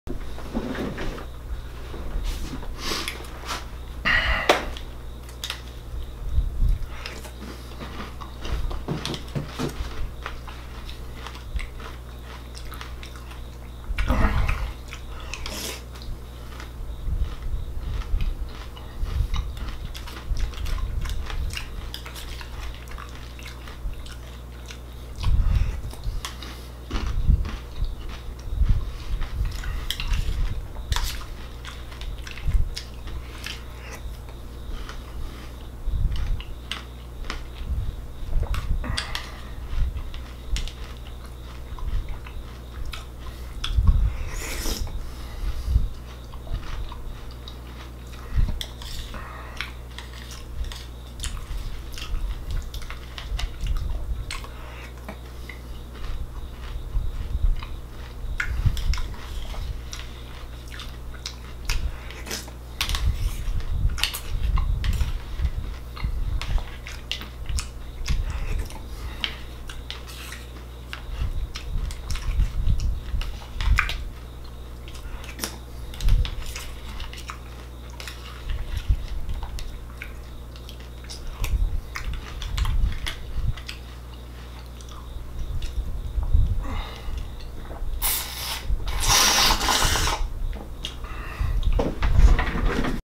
Eating A Bowl Of Cereal
On day 262 of my journey to making it big on YouTube, I eat cereal.
Second last bowl of Raisin Bran Crunch.